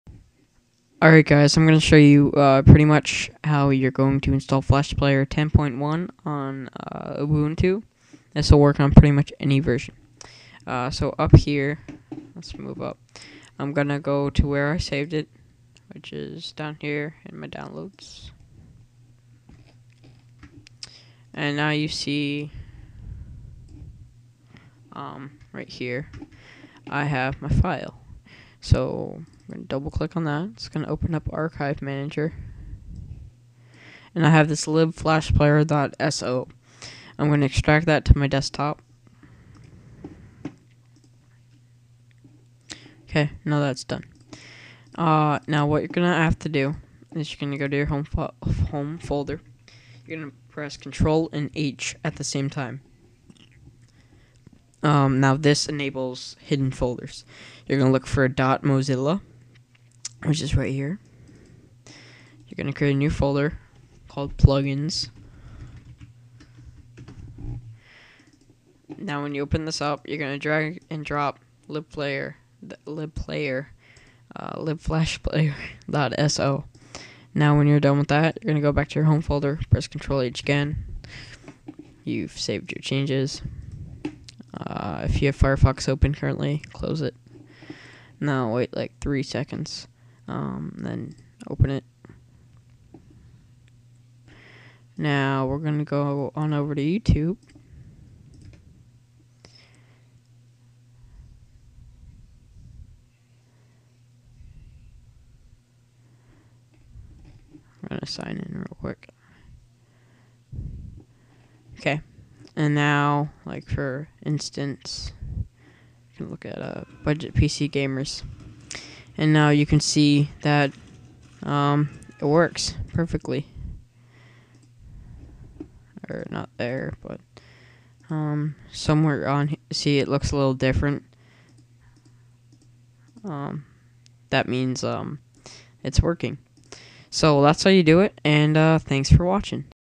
How to Install Flash 10.1 RC on Ubuntu 10.04
*NOTE* I was barely 13 when I made this video, and probably hitting puberty lol. Excuse my voice.

So yeah, this will show you how to install Flash 10.1 on Ubuntu 10.04.